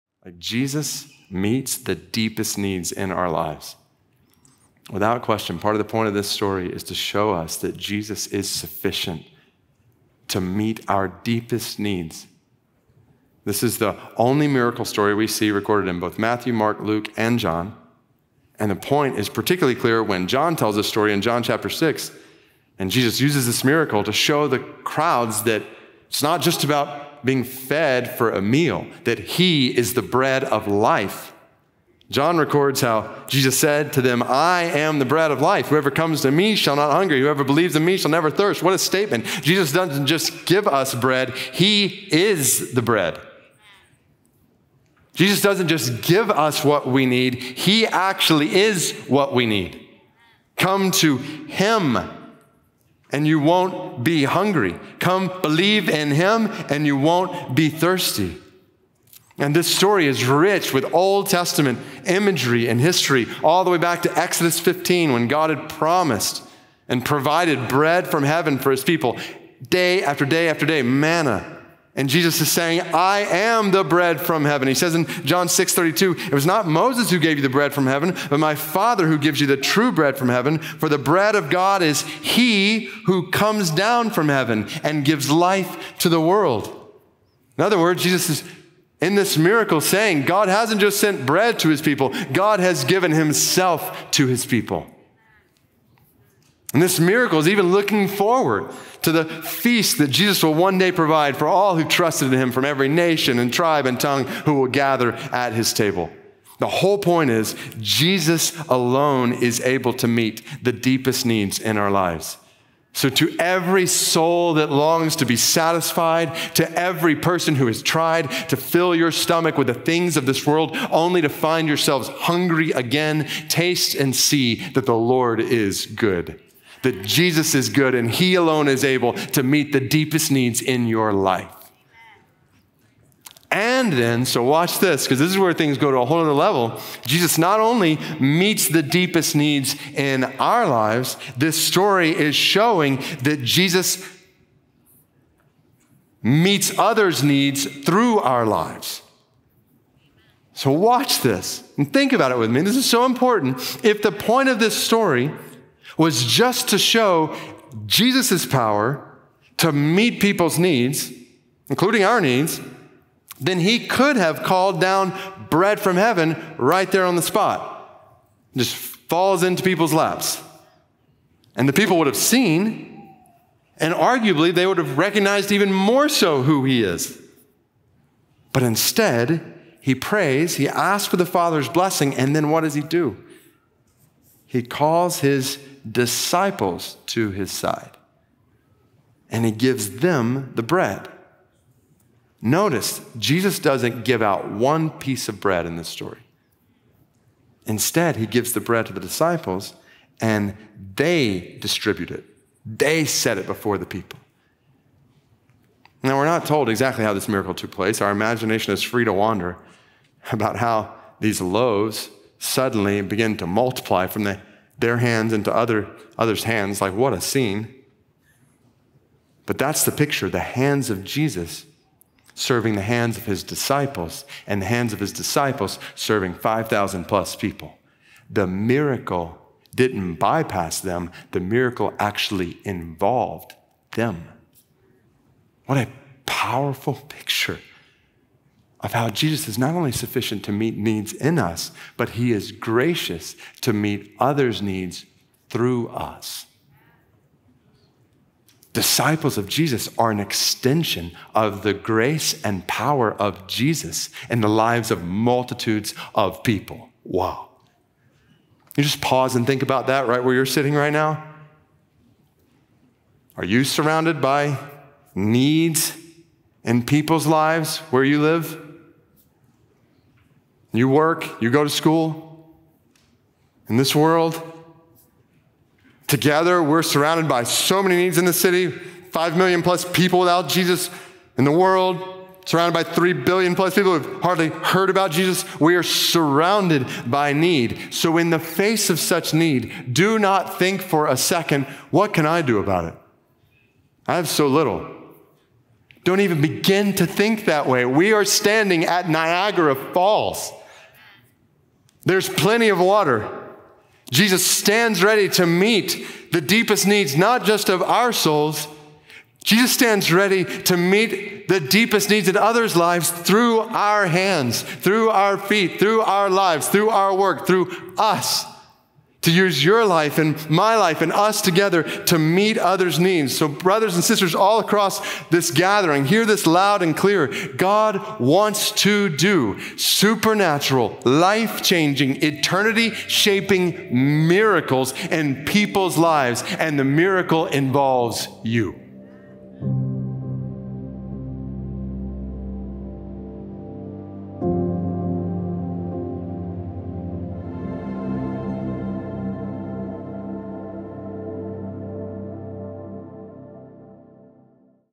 Jesus Wants to Meet Your Deepest Needs || David Platt
In this video, David Platt explains how Jesus desires to meet your deepest needs and how He desires for you to help meet the needs of others.